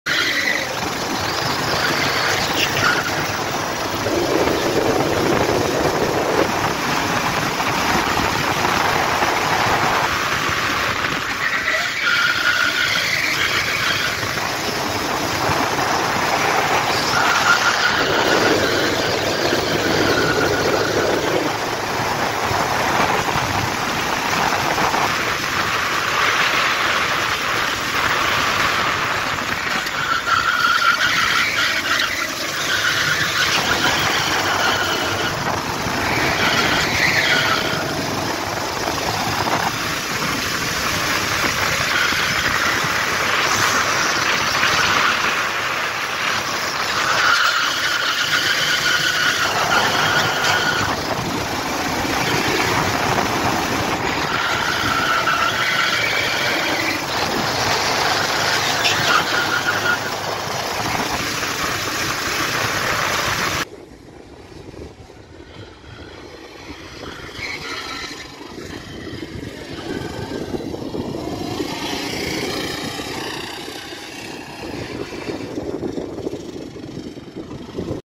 Awix Toruń - Onboard | Sodi SR4 13hp
time ~56.4s
Clean lap, but it was extremely hot and FL tyre got slightly overheated and yeah... There's potential for better time. But overall very fun track 👾